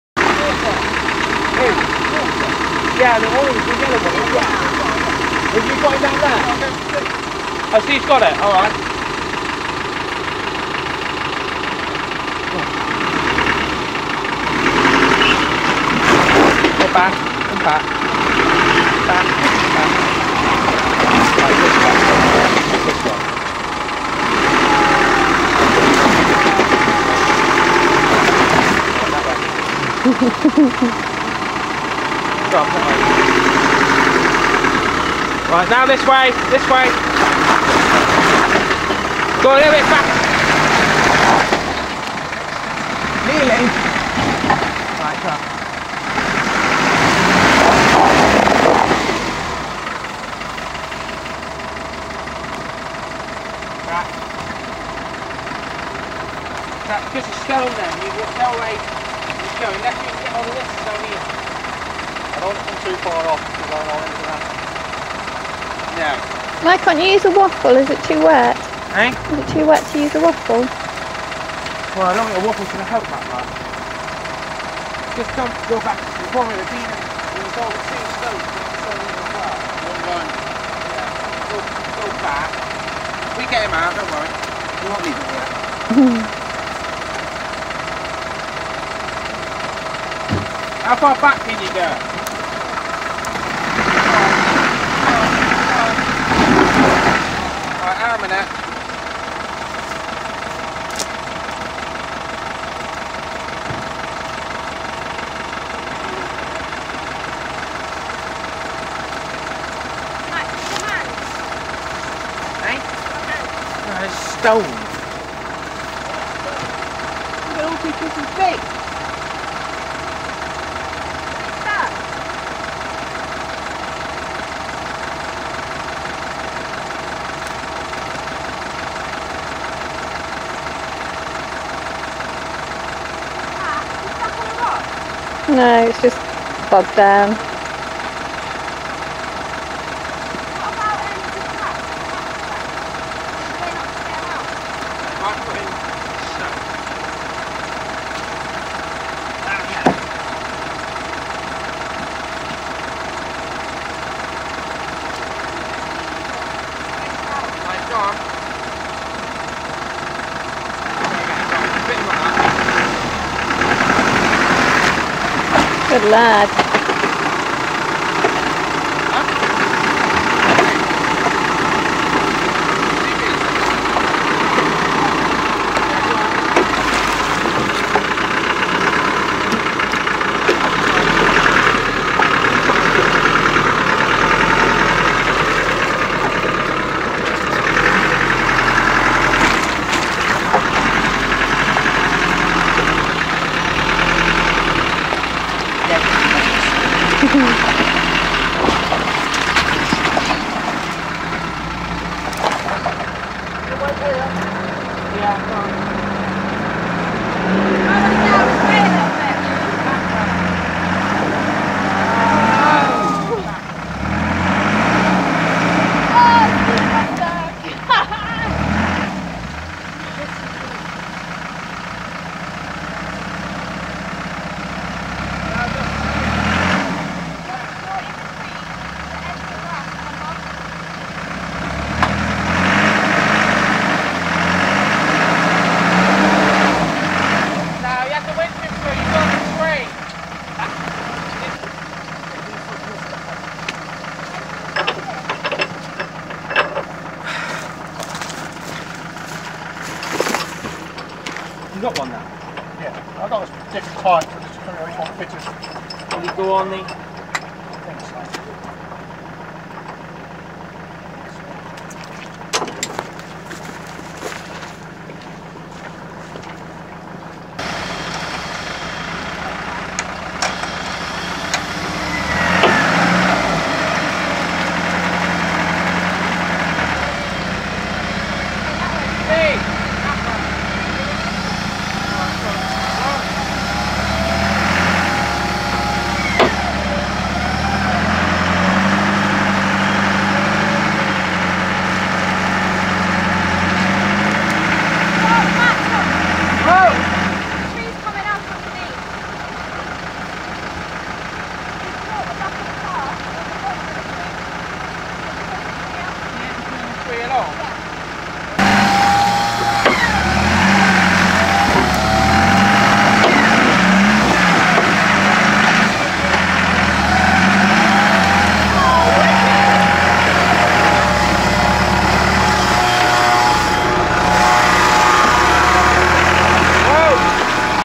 Impala Adventures - Russia - More Bog Part 2 - 26/08/2008
This track took 5 days to complete, progress was slow in places and this is an example where it got very boggy. We were a long way from civilisation so care needed to be taken not to break anything.